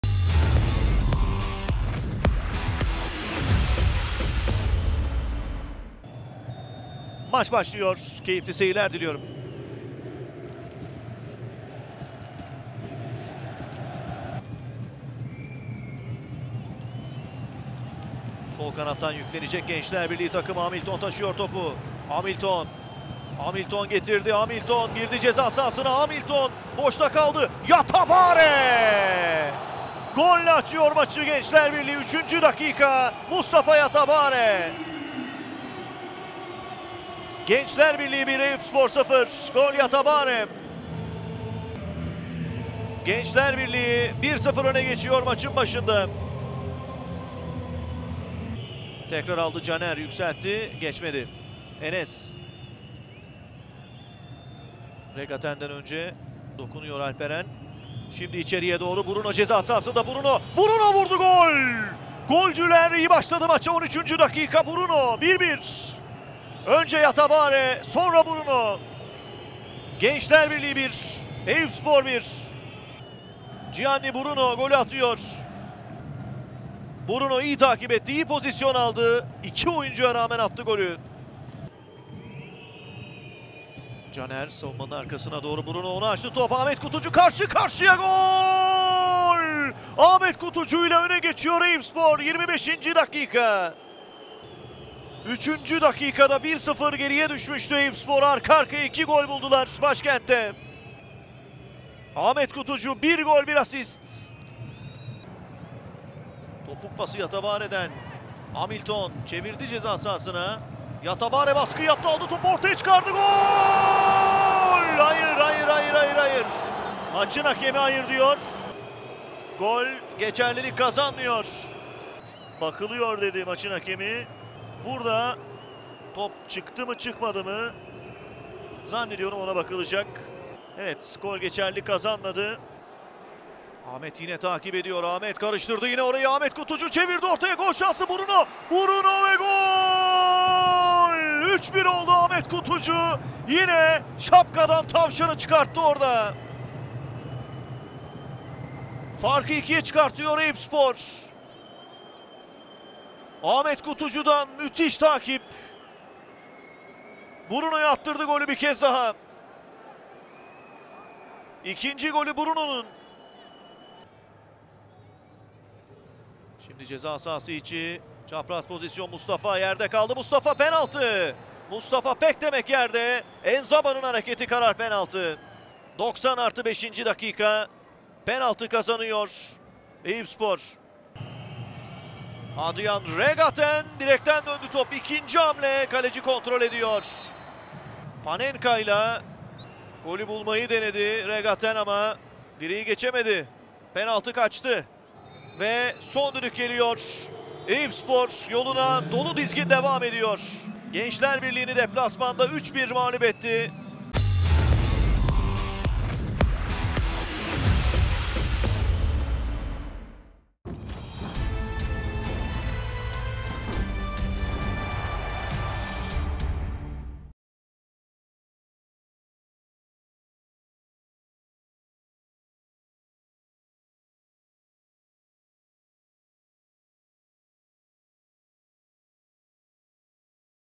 Gençlerbirliği (1-3) Eyüpspor - Highlights/Özet | Trendyol 1. Lig - 2023/24
Trendyol 1. Lig'in 5. haftasında Eyüpspor, deplasmanda Gençlerbirliği'ni 3-1 yendi.

Öte yandan Eyüpspor, 90+6. dakikada Adrien Regattin ile penaltı vuruşundan yararlanamadı.

Gençlerbirliği oyuncusu Muhammed Himmet Ertürk'ün 61. dakikada ağlara gönderdiği top, pozisyonun başlangıcında meşin yuvarlağın dışardan çevrildiği gerekçesiyle gol değer kazanmadı.

Ligde bu sezon oynadığı 5 maçı da kazanan Eyüpspor, namağlup liderliğini sürdürdü. İlk mağlubiyetini yaşayan Gençlerbirliği ise 10 puanda kaldı.